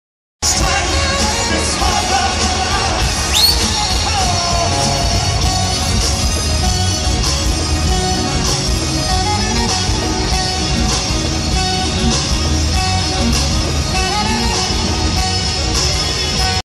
Charlie Wilson
Charlie Wilson performs at Essence Festival 2014